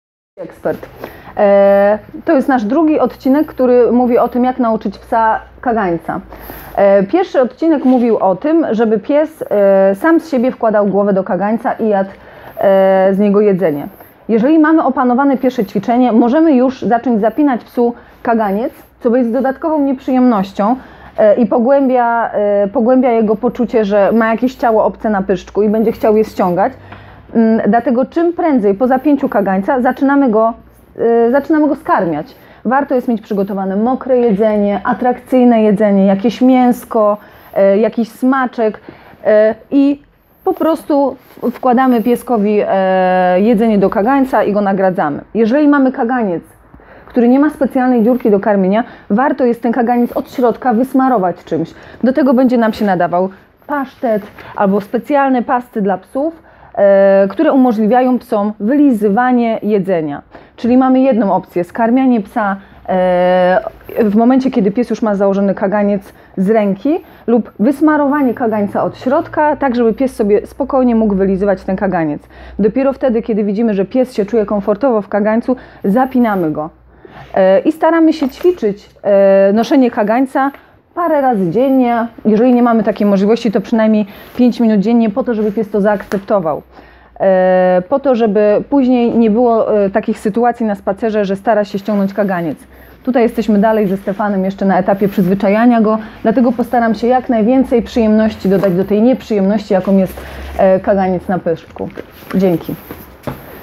Nauka noszenia kagańca - część druga
Nauka noszenia, może być przyjemna dla psa. Jak zachęcić psa do noszenia kagańca, jakie ćwiczenia ułatwią nam to naukę?